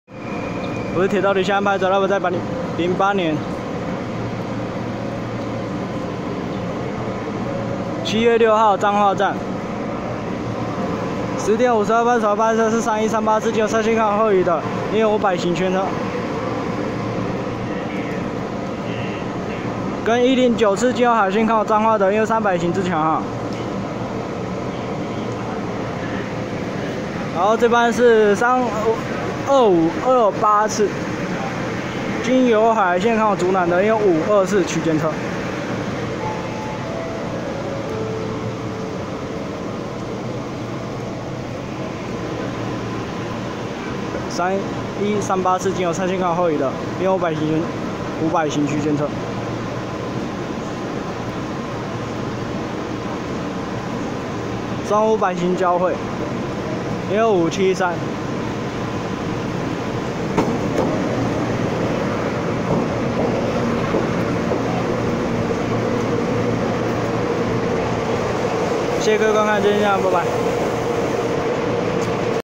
3138次經由山線開往后里的EMU573未優化+EMU556台灣燈會在屏東彩繪優化混編二組區間車
108/7/6 彰化站 10:52拍攝的3138次經由山線開往后里的EMU573未優化+EMU556台灣燈會在屏東彩繪優化混編二組區間車